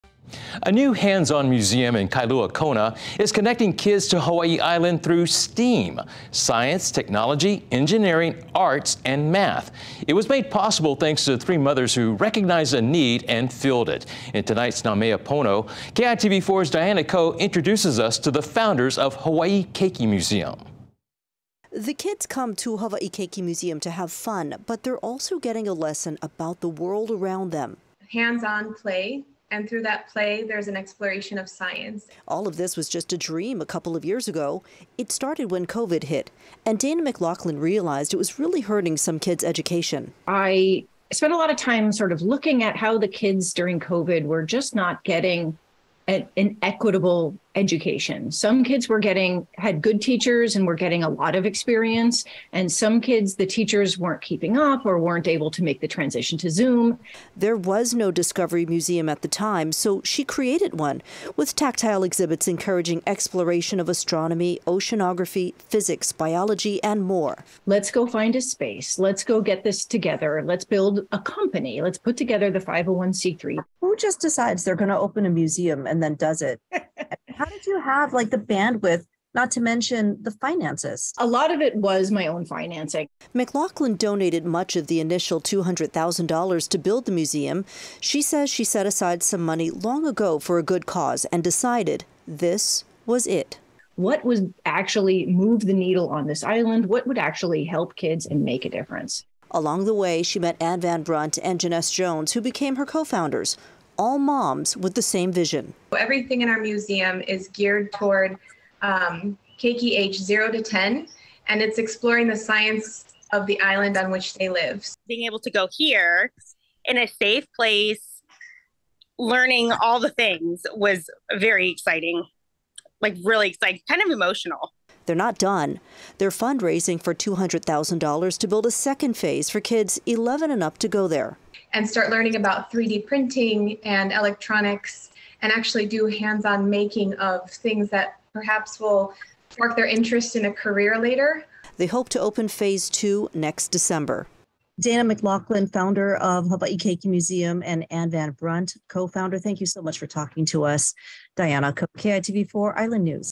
New museum for keiki comes to Big Island
A new hands on museum in Kailua-Kona is connecting kids to Hawai'i Island -- through  STEAM -- Science, Technology, Engineering, Arts and Math.